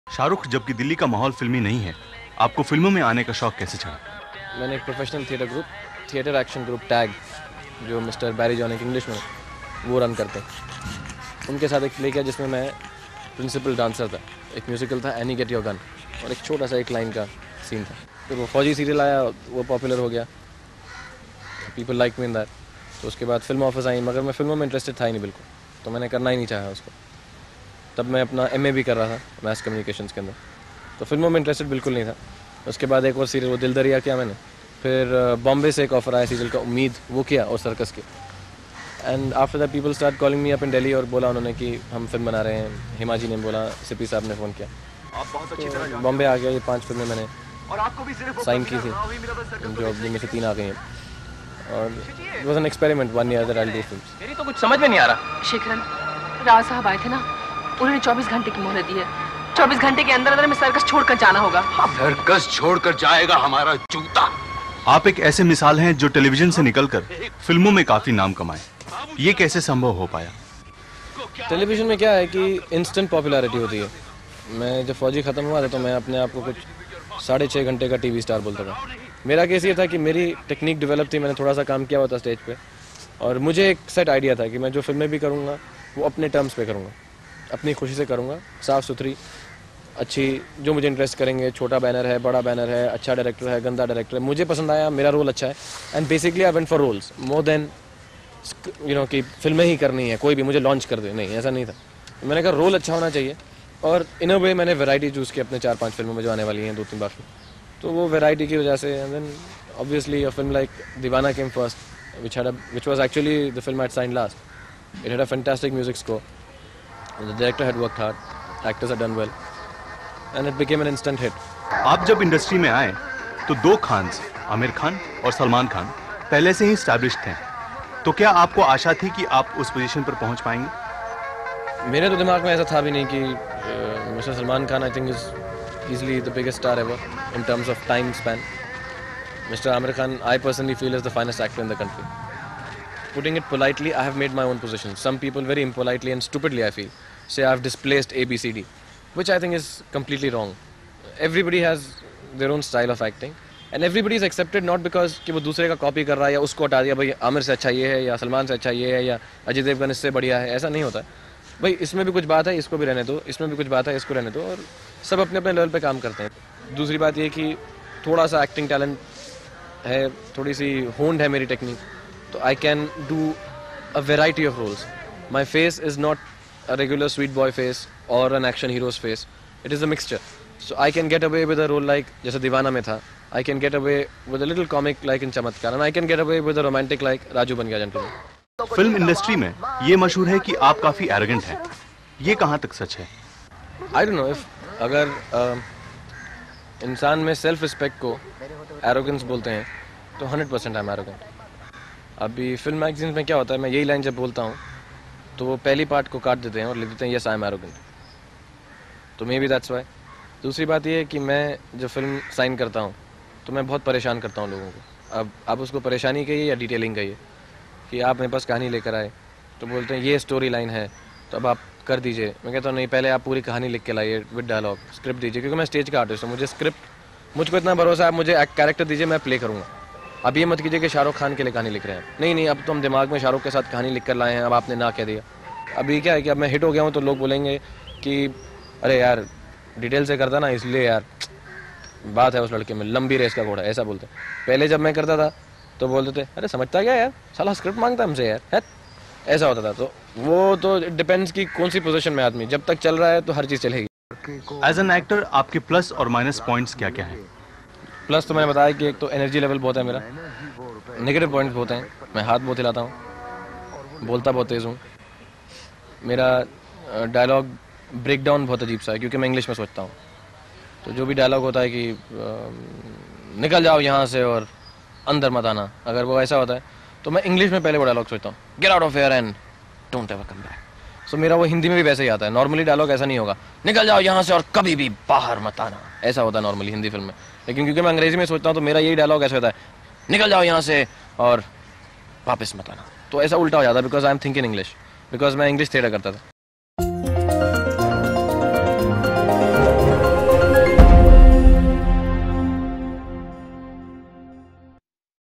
Shahrukh Khan: I Never Wanted To Do Films | Flashback Interview
Watch this throwback interview of Shahrukh Khan where he recalls his journey from theatres to television and films. He shares that he was never interested in doing films and how it eventually happened. ShahrukhKhan About Lehren